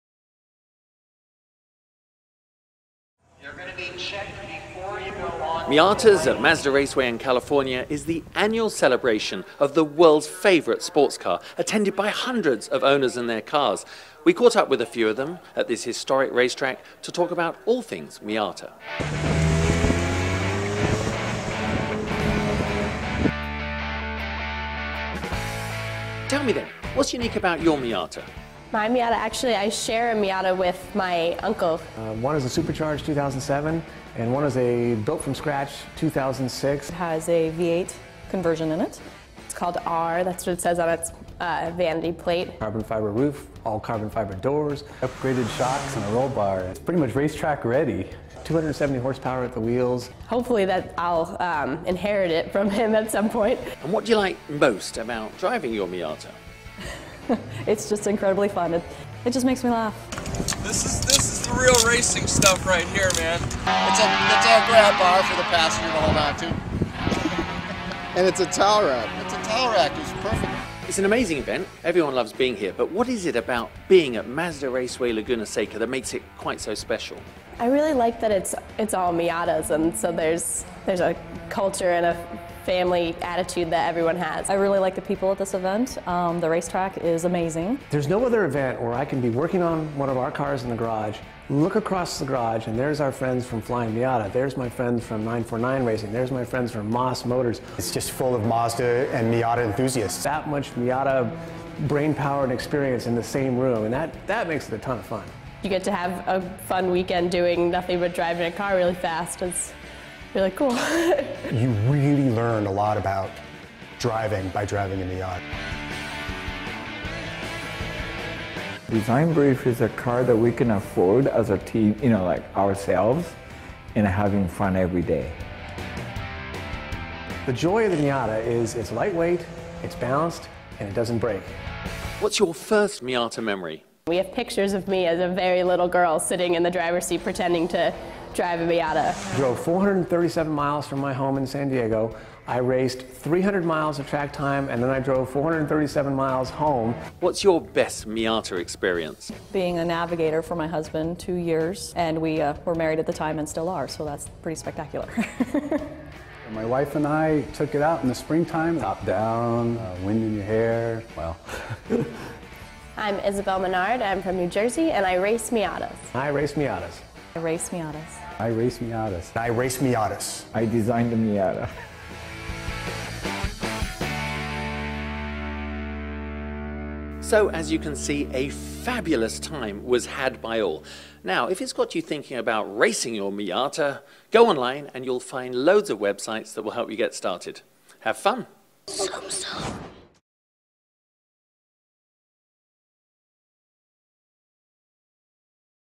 Mazda Miatas MX-5's at Mazda Raceway Laguna Seca
Miata owners at the Miatas at Mazda Raceway event, at the historic Mazda Raceway Laguna Seca racetrack, and talk about all things Miata.